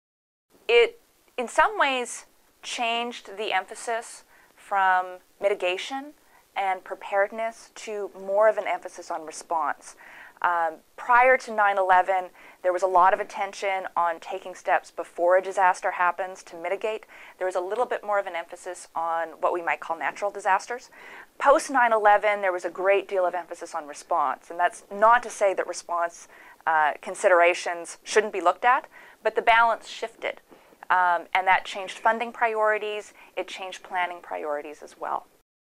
Disaster Preparation- Part 1
Tricia Wachtendorf, Associate Professor at the University of Delaware Disaster Research Center, explains how 9/11 changed how we prepare for disasters.